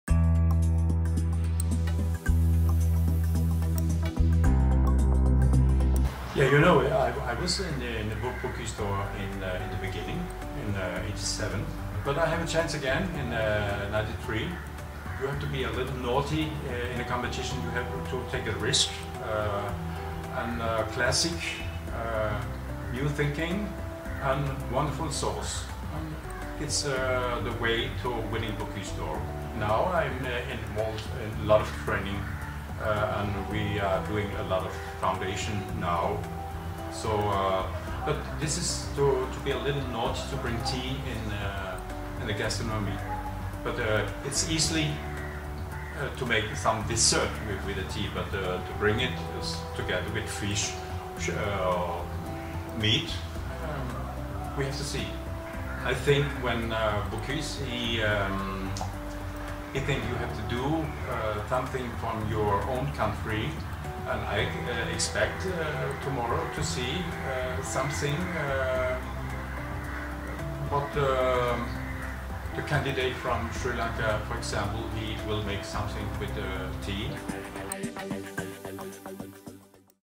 Chief Judge of the Sri Lanka Bocuse d'Or Culinary Challenge Chef Jens Peter Kolbeck
Chief Judge of the Sri Lanka Bocuse d'Or Culinary Challenge Chef Jens Peter Kolbeck shares his experiences on winning the coveted Bocuse d'Argent in 1993 and his thoughts on tea and gastronomy.